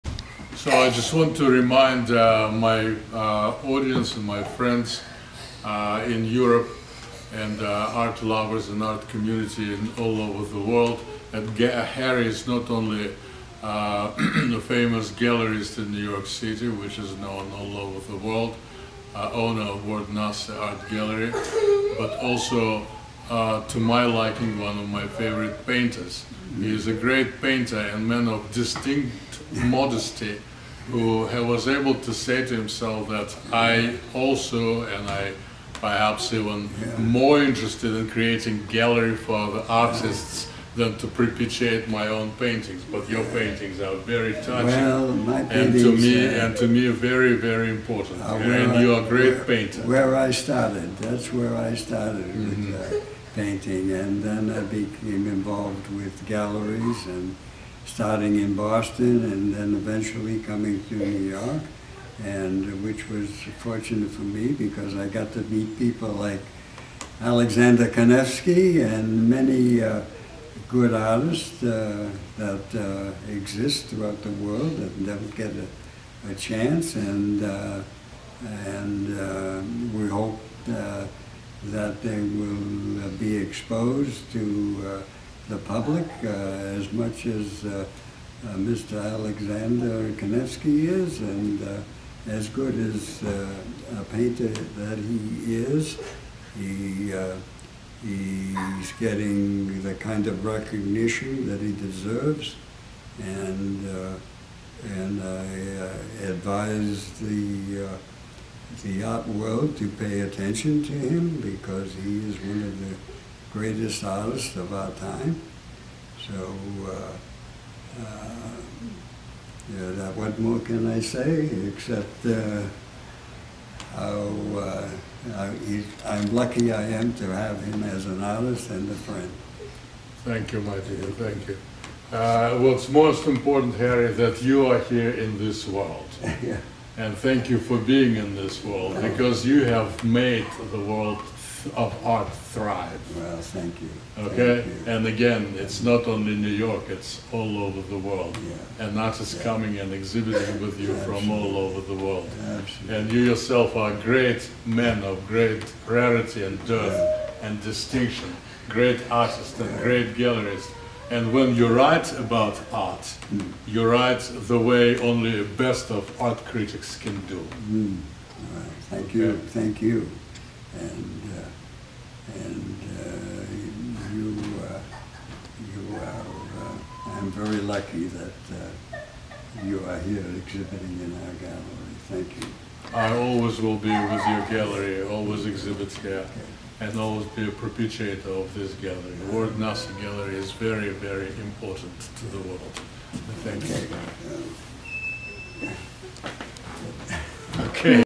Artist Alexander Kanevsky's interview by Harry Nasse at SoHo New York Ward-Nasse Gallery
Alexander Kanevsky speaks with Harry Nasse in front of Alexander's giant panel with "Swan" and "Parrot" in New York City SoHo Ward-Nasse Gallery. Part 3,